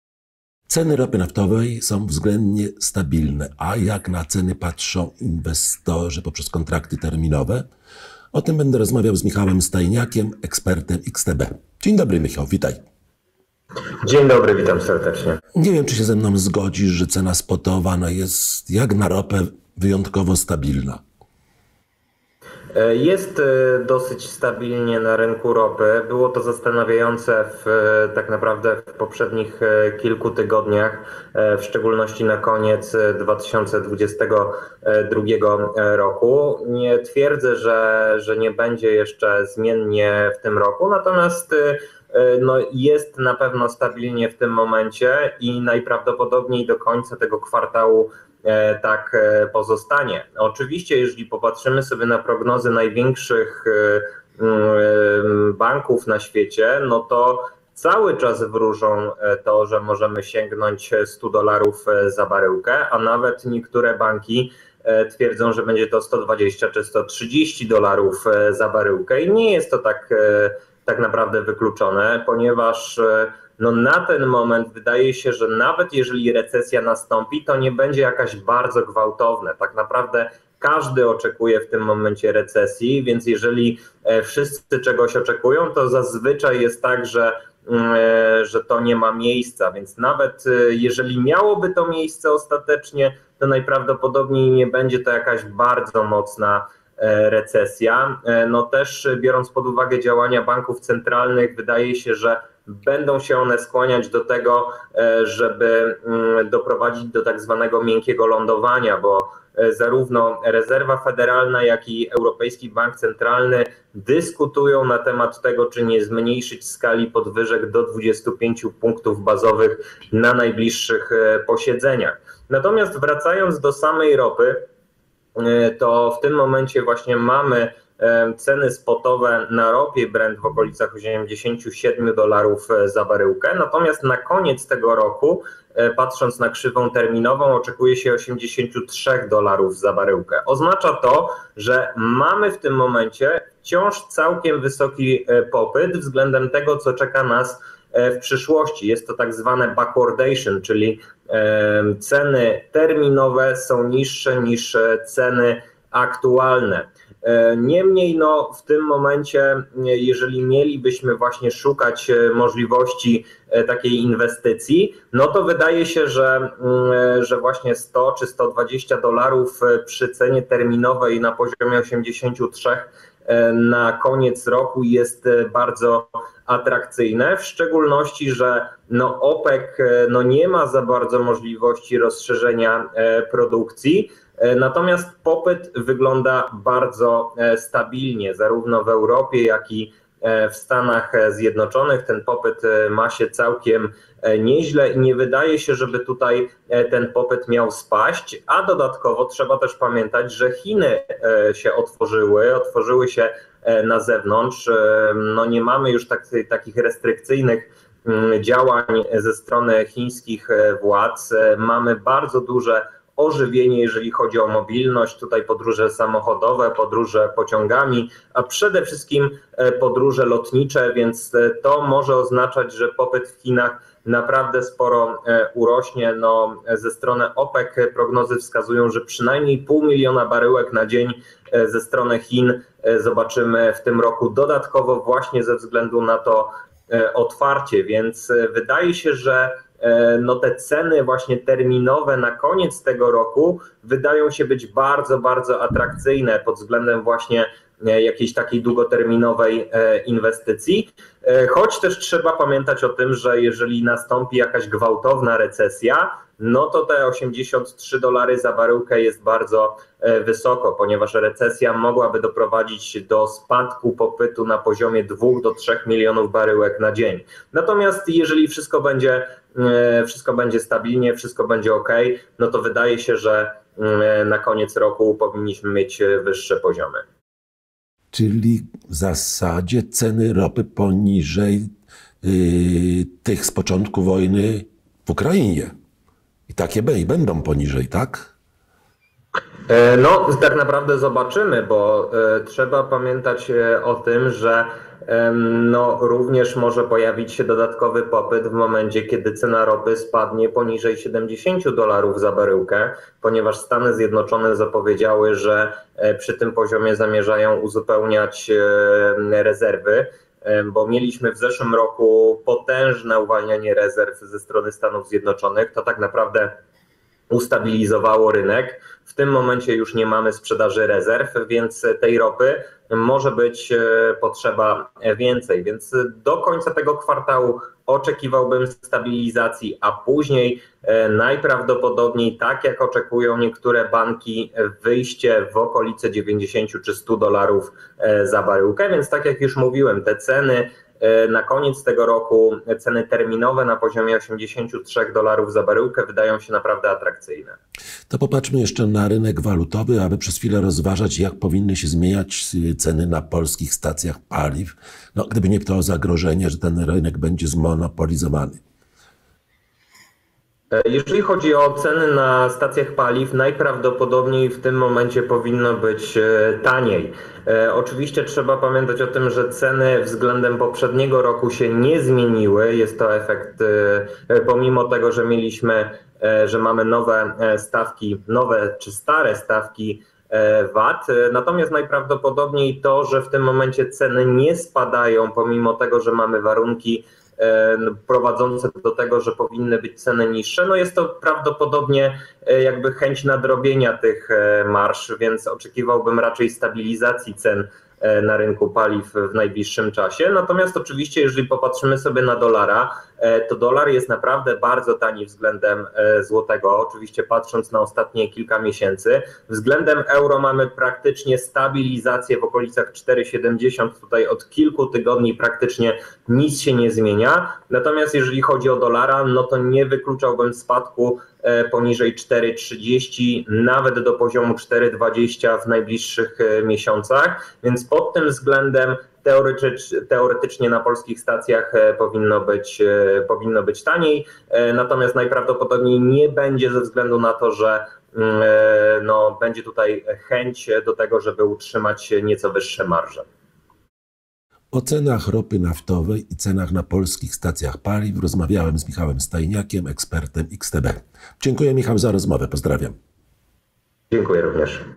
Ceny ropy naftowej są zaskakująco stabilne
Sytuacja jest o tyle dziwna, że ceny ropy naftowej są dość stabilne, pomimo dużej niepewności związanej z zagrożeniem recesją i wojną w Ukrainie, a ceny terminowe na ropie brent są niższe od cen aktualnych.

W listopadzie 2022 r. cena ropy brent zbliżyła się bardzo do poziomu 100 USD za baryłkę. W połowie listopada spadła poniżej 90 USD i tego poziomu nie przebija. Nie są to duże zmiany, bo przecież ropa naftowa jest najbardziej politycznym surowcem, a więc o szczególnie dużych wahaniach cen.

- Ceny ropy są dość stabilne, co było szczególnie zastanawiające w ostatnich tygodniach 2022 r. i najprawdopodobniej do końca I kw. br. tak pozostanie – mówi w rozmowie z MarketNews24 Michał Stajniak, ekspert XTB.

Prognozy największy banków inwestycyjnych nadal zdominowane są przez przewidywania, że ceny ropy osiągną poziom 100 USD za baryłkę. Są banki, które twierdzą, że cena wzrośnie nawet do 120-130 USD, a to dlatego, że jeżeli jednak dojdzie do recesji, to ta nie będzie ani gwałtowna, ani głęboka. Najważniejsze banki centralne starają się zadbać, aby gospodarka miała zapewnione „miękkie lądowanie”. Dlatego obniżą podwyżki stóp procentowych bądź powstrzymają się przed nowymi podwyżkami (tak, jak ostatnio postąpił Bank Kanady).

Przy założeniu, że takie scenariusz jest dominujący inwestorzy nie spodziewają się obniżenia popytu na ropę naftową. Popyt obecnie jest bardzo stabilny, w Europie i w USA.

- Sytuacja jest o tyle ciekawa, że ceny terminowe na ropie brent są niższe od cen aktualnych (spotowych) – dodaje ekspert XTB. – Inwestowanie w ropę staje się atrakcyjne przy cenie 100-120 USD na kontraktach terminowych, ponieważ na koniec 2022 r. cena ropy brent znajdowała na poziomie 83 USD.

Przy długoterminowym inwestowaniu tym bardziej należy uwzględnić ożywienie w chińskiej gospodarce, związane z odejściem od polityki „zero covid”. I także to, że jeżeli jednak cena ropy będzie niska (zbliży się do 70 USD), to Stany Zjednoczone przystąpią do odbudowywania rezerw.

Jak światowa sytuacja dotycząca ropy wpłynie na ceny na polskich stacjach paliw?

- To że ceny nie spadają, choć powinny być niższe bo dolar okazuje się bardzo słaby wobec złotego, jest prawdopodobnie spowodowane próbą nadrobienia marż, dlatego spodziewam się w najbliższych tygodniach stabilizacji cen na stacjach paliw – ocenia M.Stajniak z XTB. – Cena dolara w najbliższych miesiącach może spaść nawet do 4,20 zł, a więc powinno być taniej, co nie oznacza jednak, że tak się stanie.